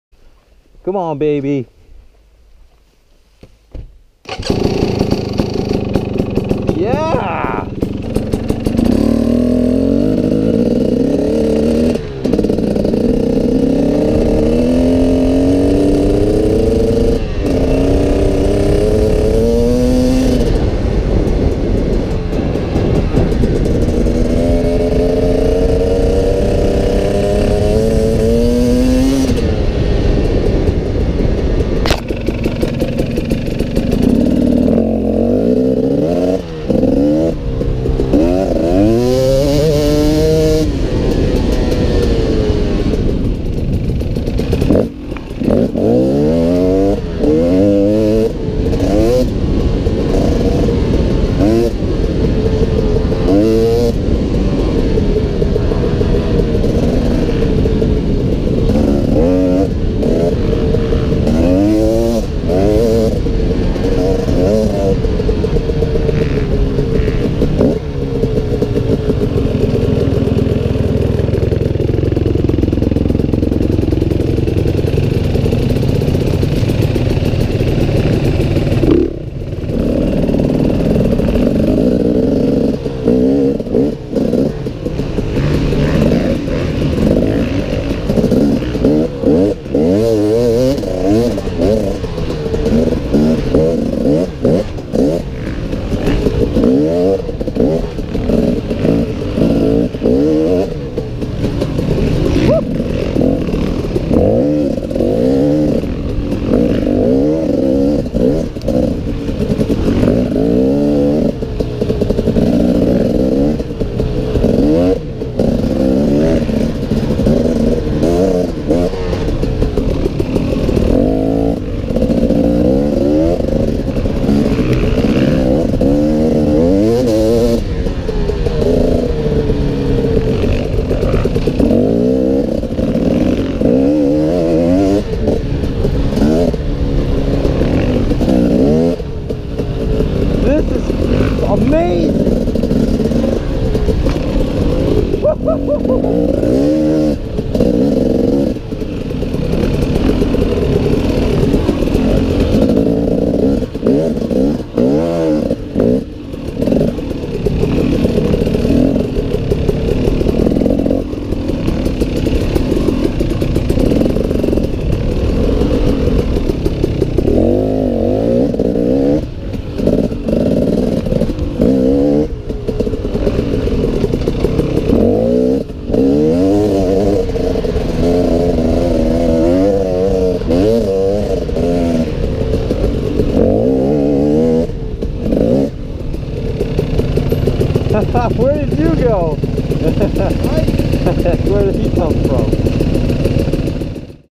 KTM Talk Input 2017 KTM 250 XC W Jetting
Looking for some advise on how you think the bike sounds?  The bike has an FMF Turbine Core 2.0 exhaust.  V4Force Reeds, Rekluse, and completely OEM jetting - I haven't touched it once, not even the air screw.